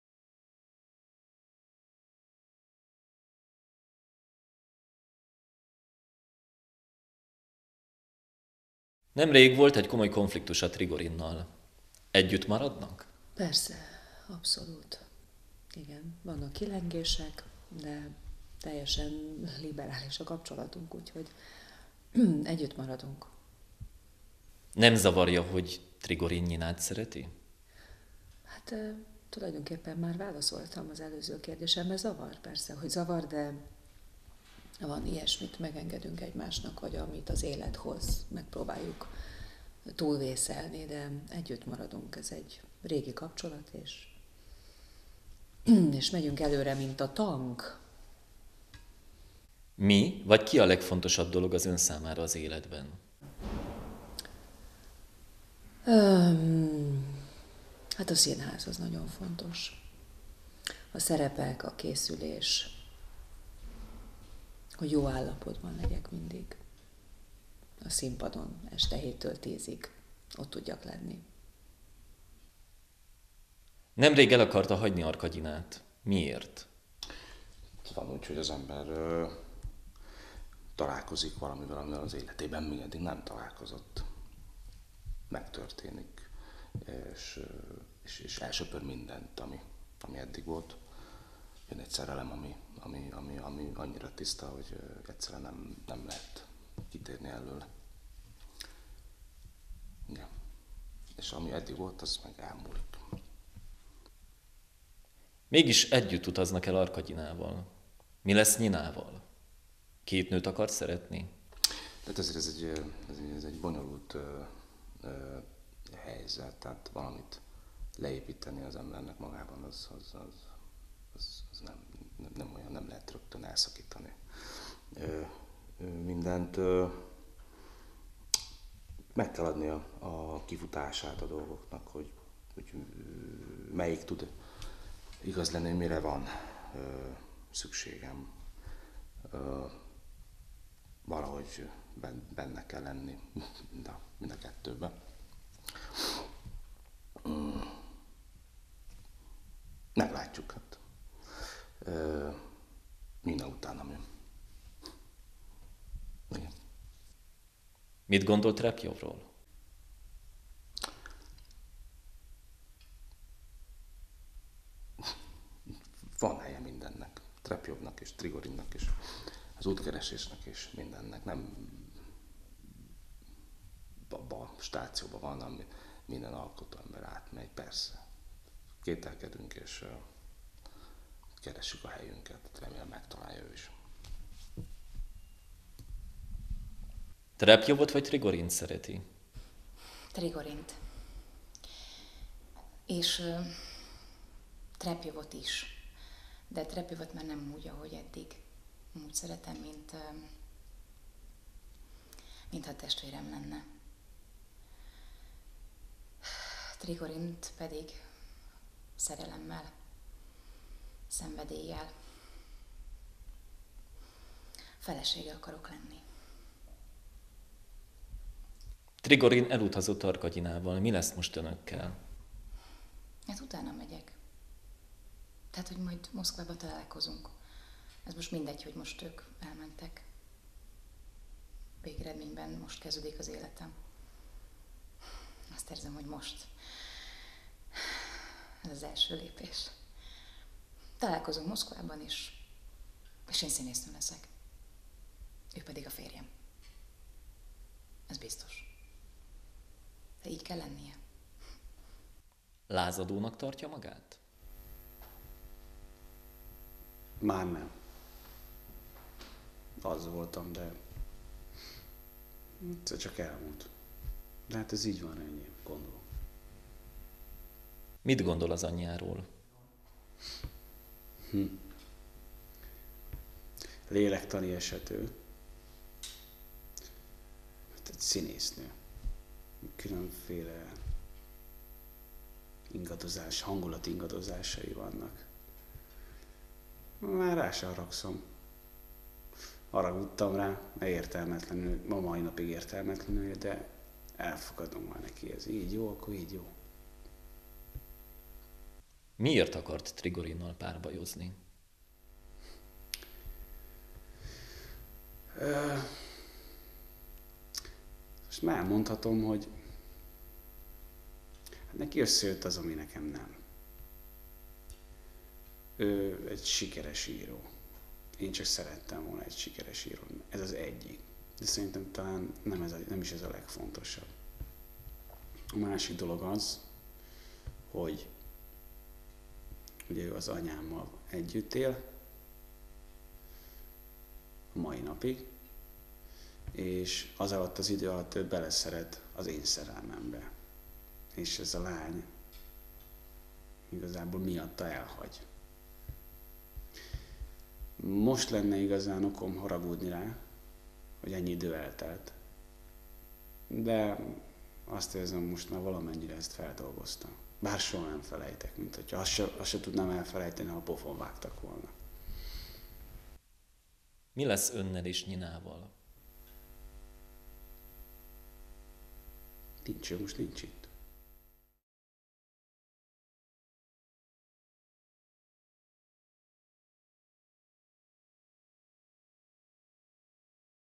Sirály avagy 80 kiló szerelem, interjúk, Jászai Mari Színház, Népház
Anton Pavlovics Csehov
Sirály avagy 80 kiló szerelem
színjáték

Fordította: Háy Gyula
Rendező: Rába Roland

Bemutató: 2015. február 28.